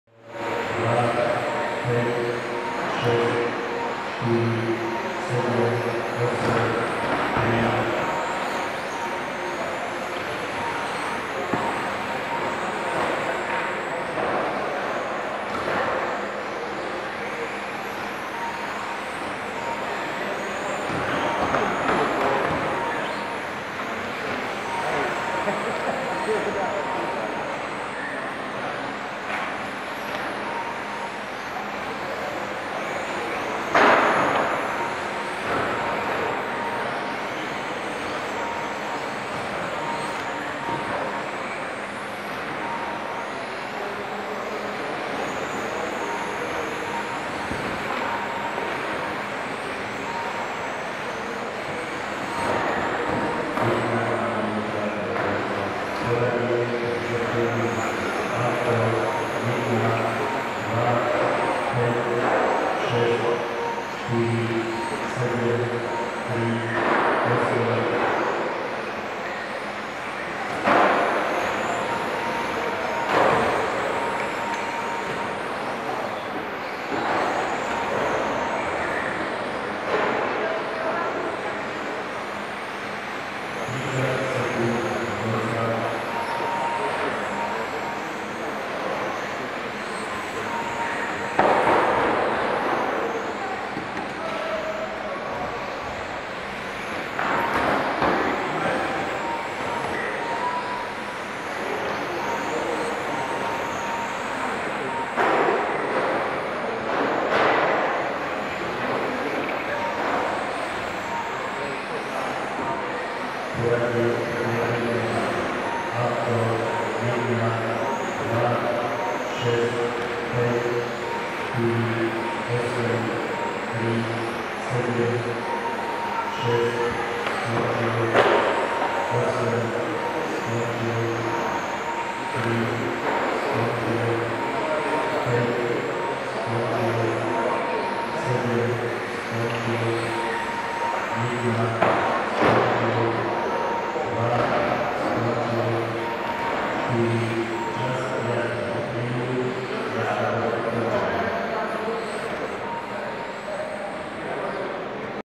Štefanský pretek 1:10 Banská Bystrica 26 12 2014
Záznam z časti preteku RC modelov automobilov DTM v mierke 1:10 4WD v Banskej Bystrici na Štefana 2014.